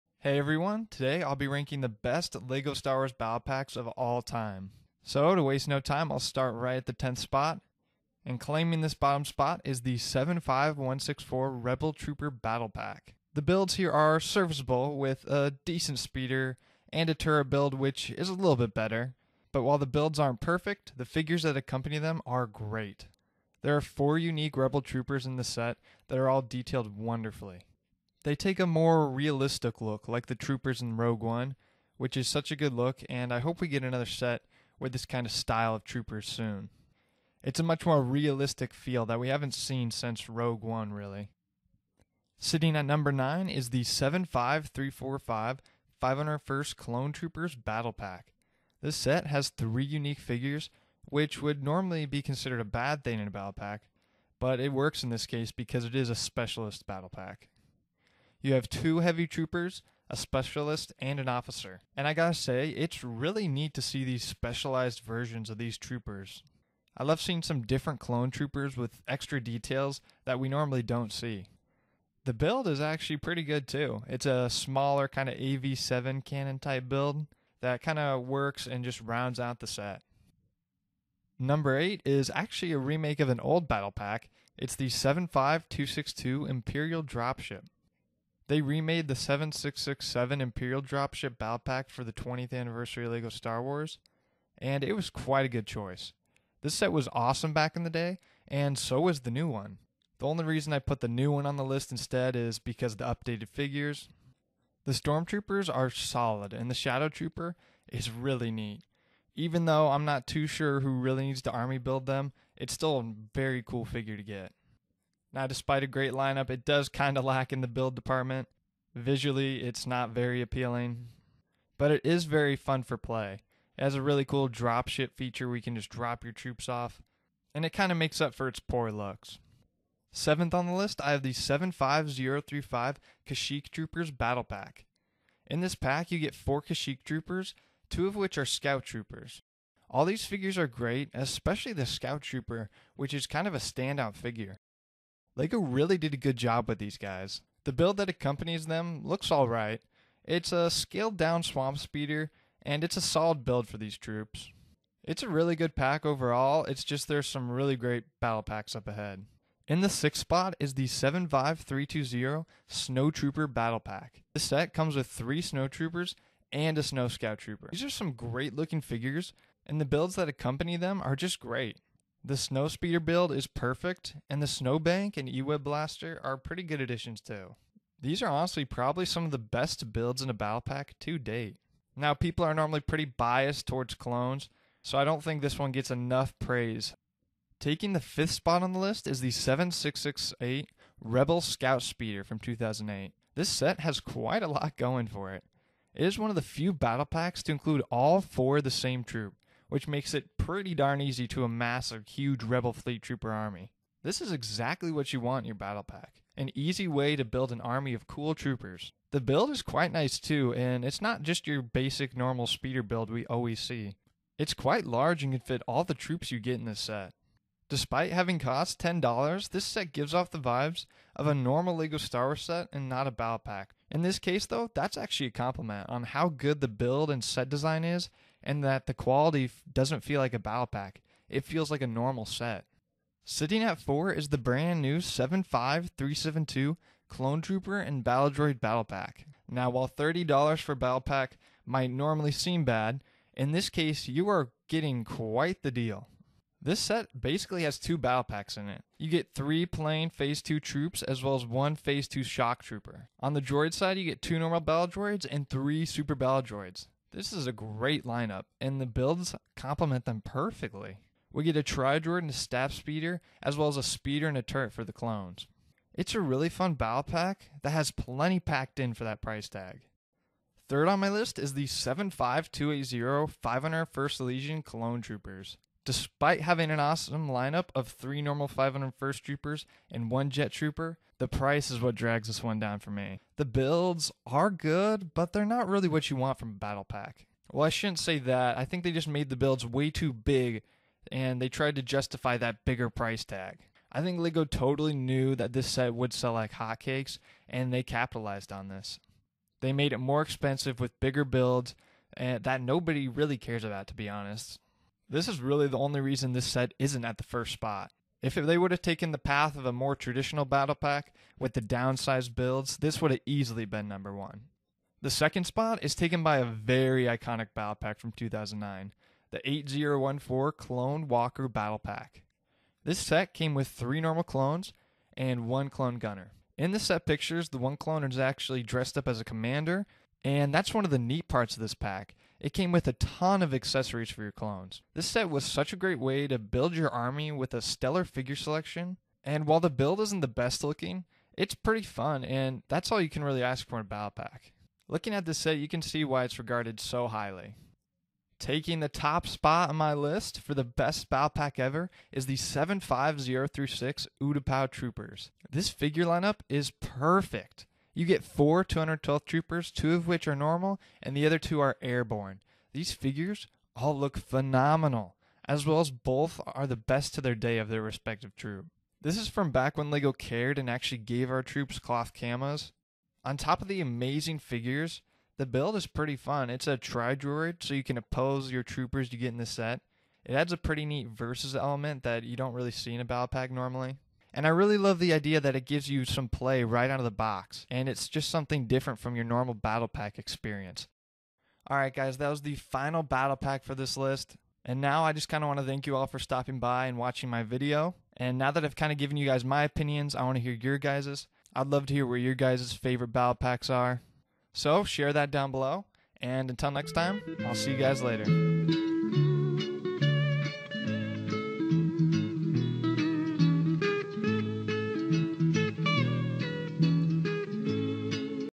Top 10 LEGO Star Wars Battle Packs of All Time
Everyone loves a good LEGO Star Wars battle pack and over 25 years of LEGO Star Wars we have had plenty so in this video I rank my top 10 favorite battle packs ever. Battle packs have changed a lot over the years and its pretty cool to go back and compare all of them to one another. As a big clone wars fan this list might be a little favored but I tried to be a little more objective in my top 10.

00:00 Intro
00:10 Top 10 LEGO Star Wars Battle Packs
06:36 Outro